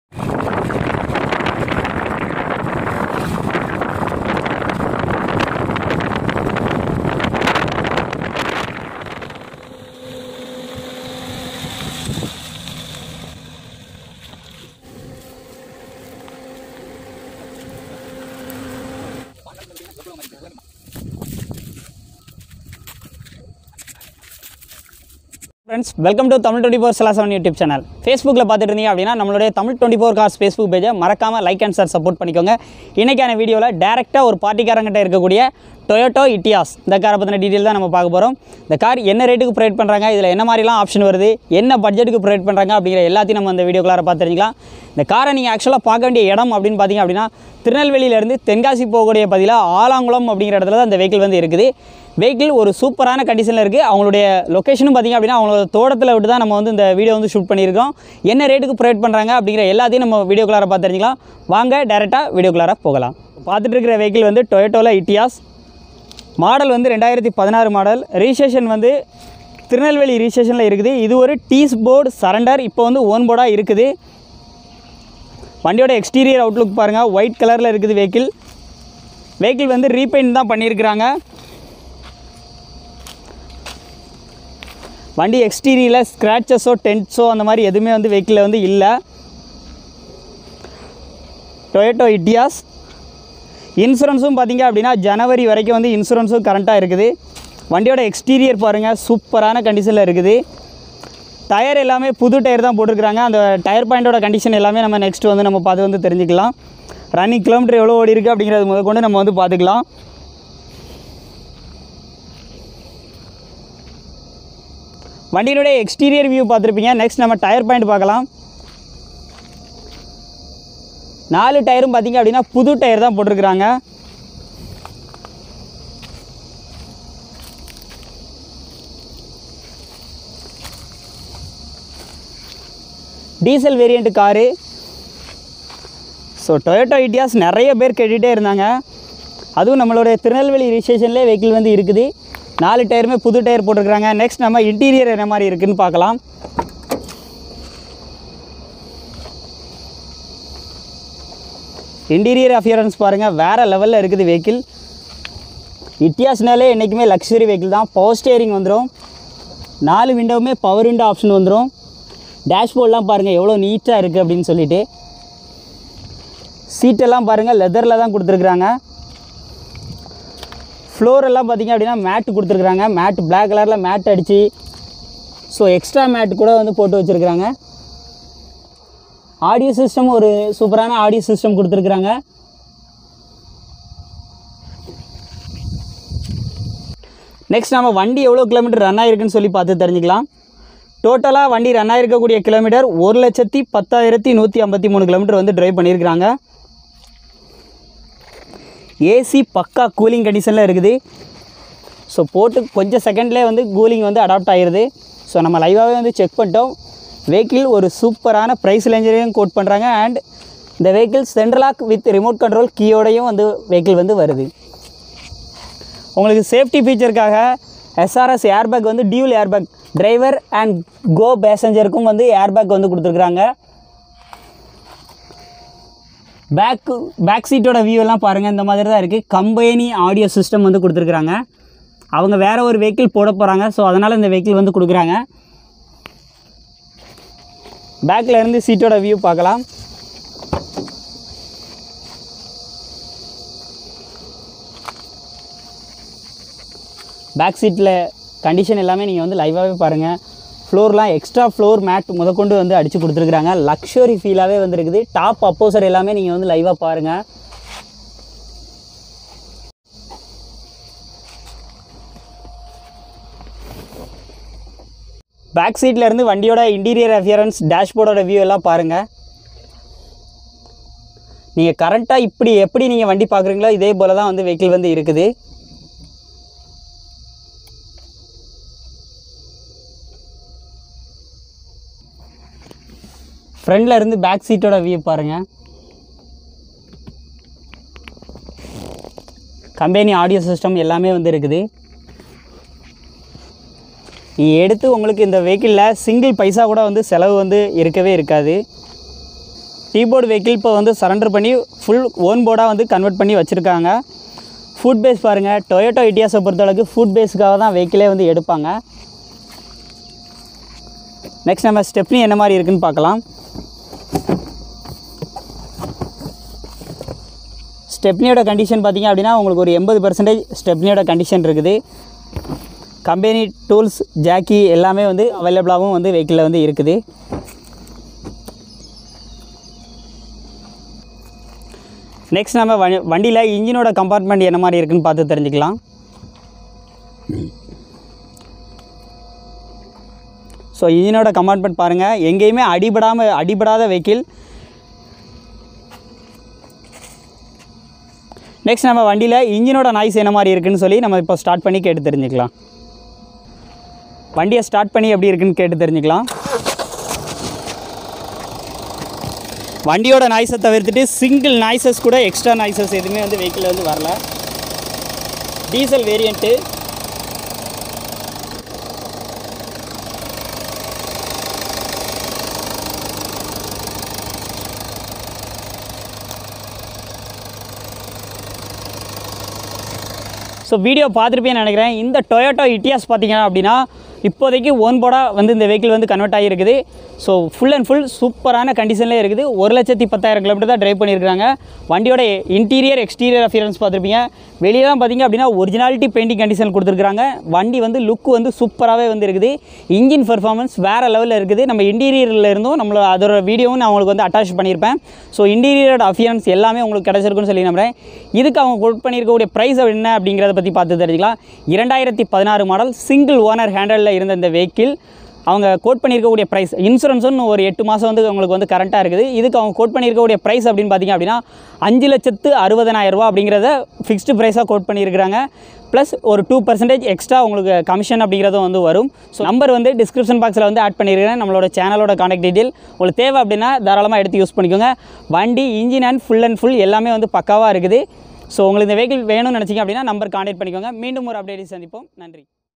TOYOTA ETIOS single owner கார் விற்பனைக்கு | தமிழ் 24/7
Price : 560000-/negotiable

Toyota etios Go
2016
Single owner
T board surrender
Re- register
105000km driven
Dual airbag
Rexin seat
Ac, ps, pw
Engine smooth condtion